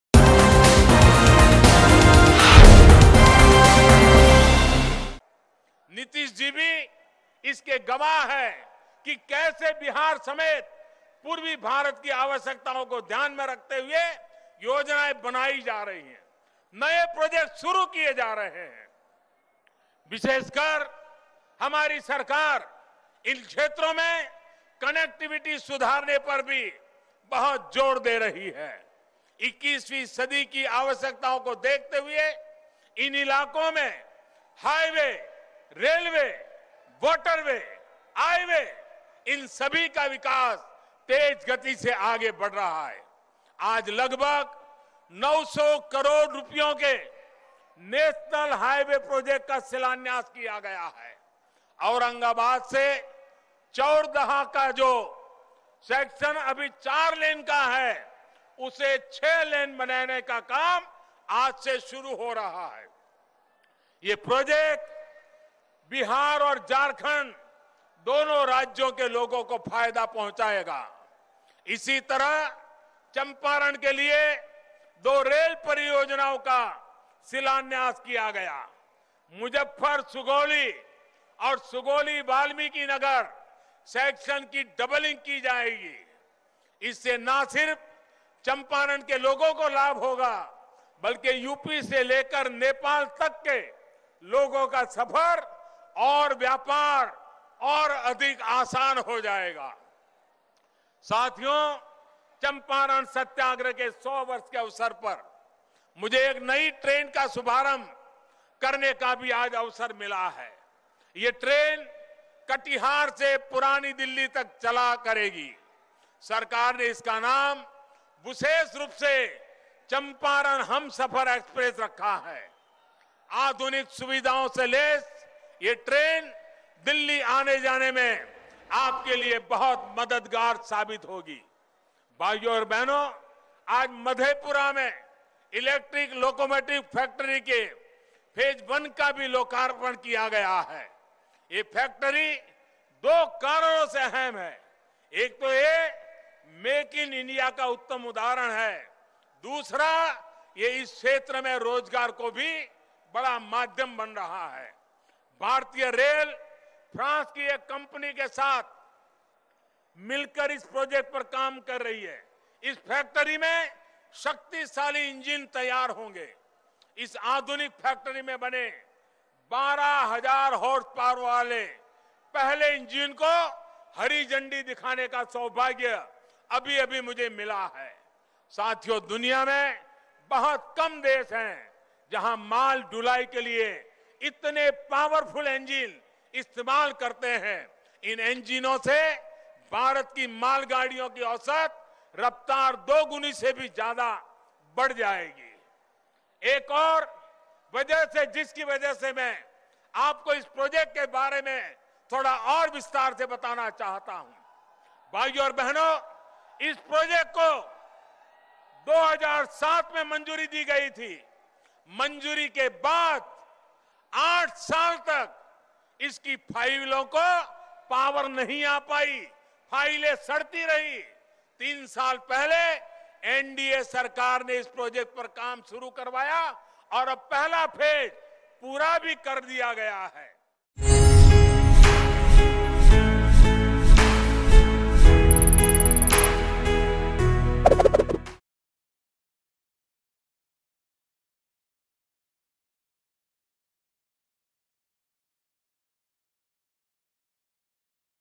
PM Modi Launches Host Of Railway Projects In Bihar | Mango News
Prime Minister Modi also laid the foundation for the doubling of railway lines of Muzaffarpur-Sugauli-Valmikinagar division. He also launched the commissioning of an electric locomotive factory at Madhepura through remote control. This will create employment opportunities in the region.